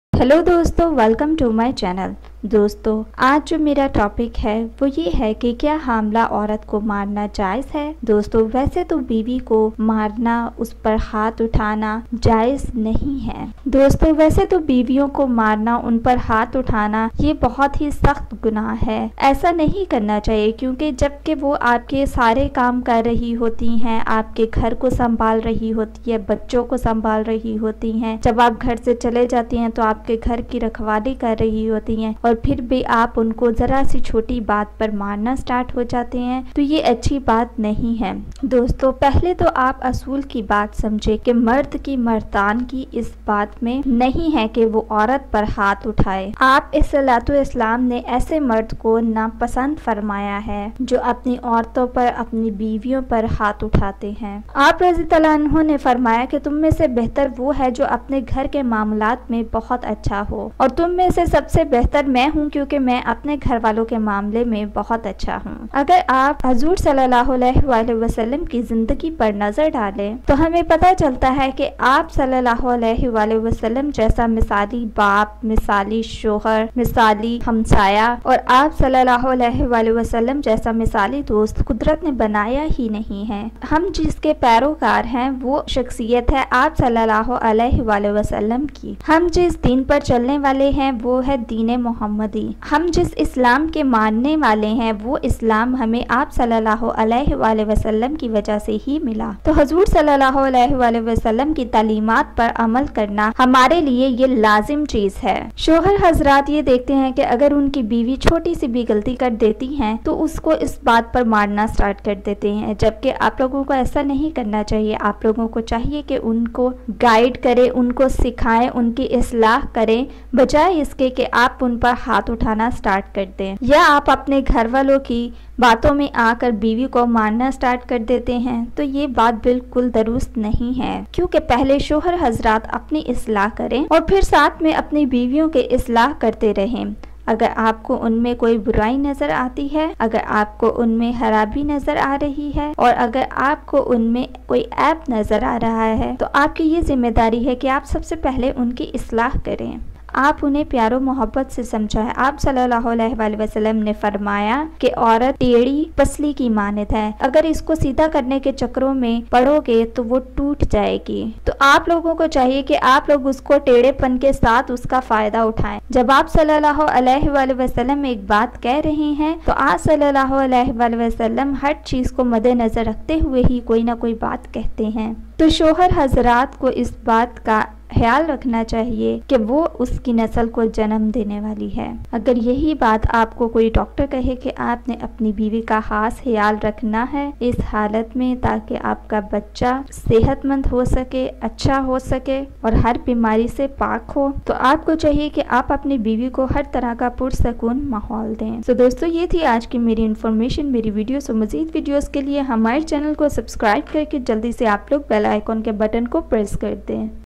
Kya Pregnant BV Ko Marna Jaiz Hai | Main Biwi Ke Huqooq in Urdu and Hindi
Dosto aj ki is video men main ap ko btao ge k kya hamala bv ko marna jaiz hai. Aj ki is video ko woh mard hazarat zaror dkhyn jo apni hamla biwiyo k sath acha salooq nai krty hai un ko choti choti btao py marna start ho jty hai. Dosto asy mard asy shohar parhy likhy ho k b jahil he smjy jty hai.